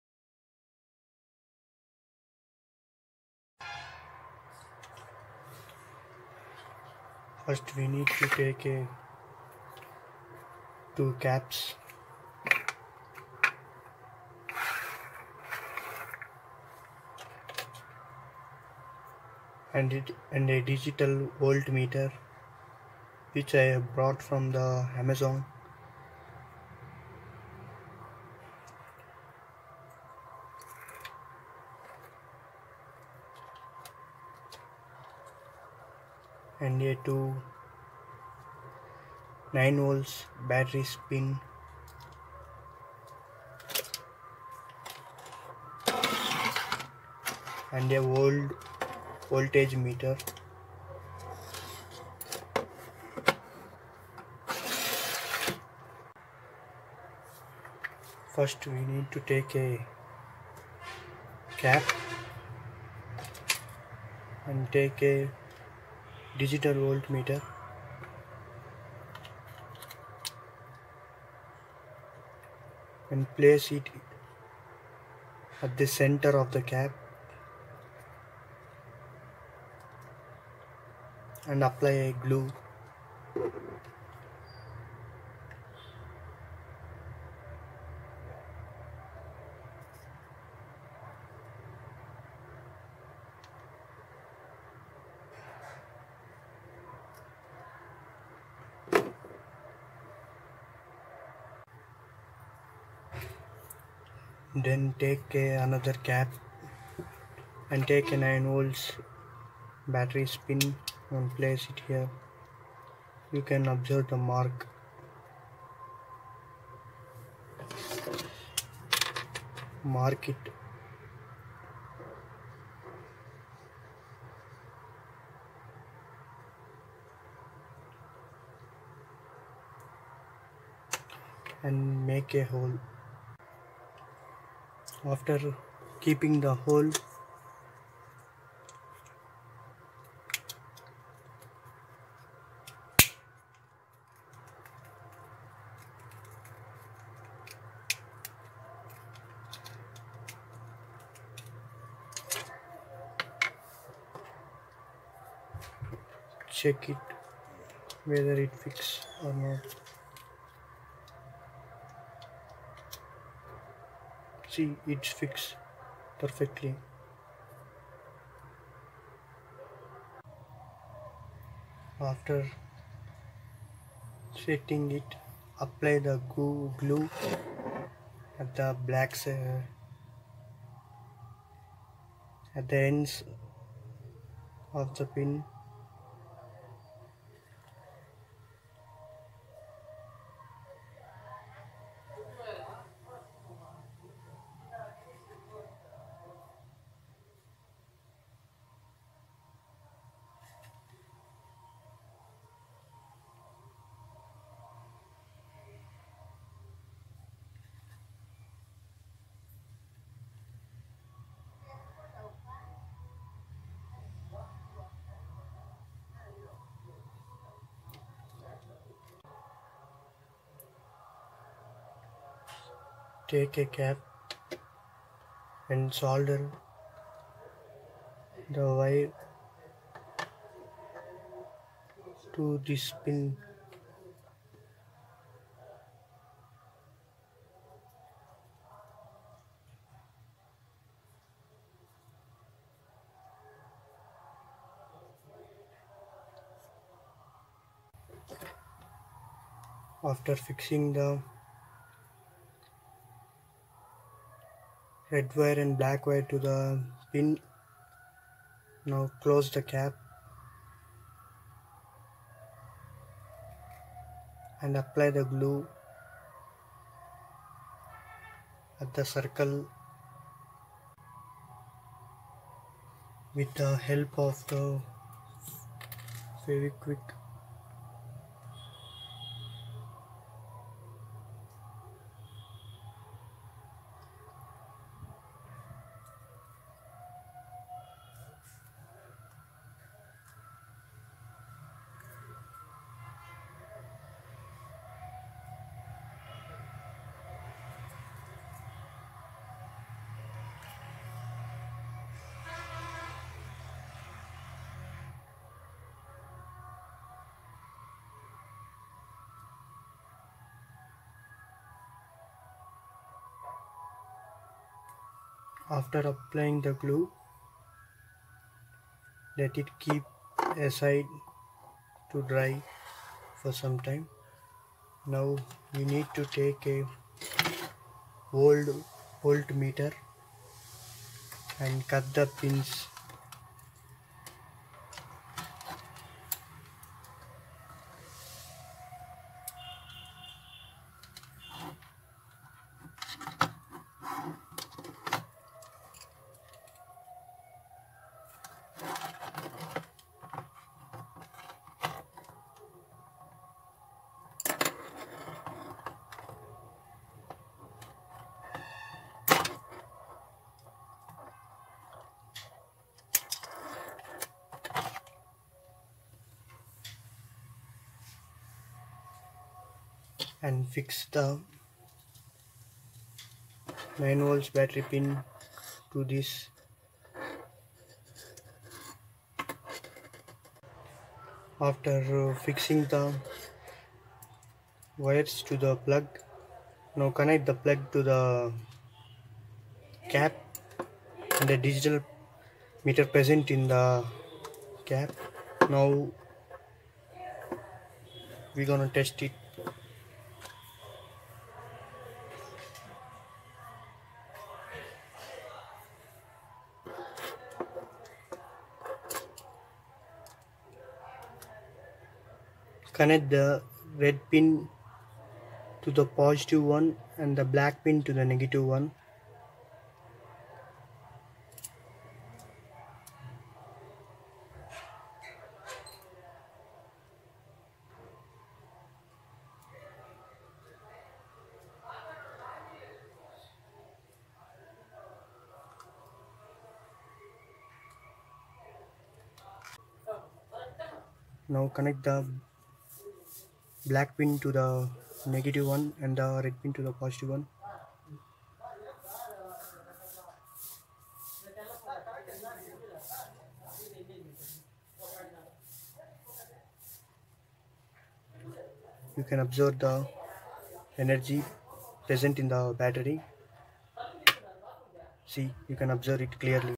How to make a DIY HOME MADE MINI VOLT METER
home made voltmeter
eHUB DC Voltmeter with Reverse Protection and 0.56 Inch Red LED Digital Display (DC 4.5-30 V)